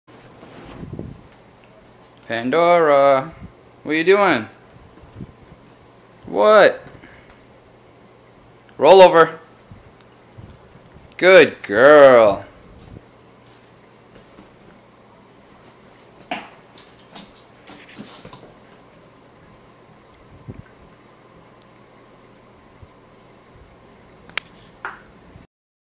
Min Pin Pandora Learned Roll Over In One Day
She learned this yesterday and does not forget at all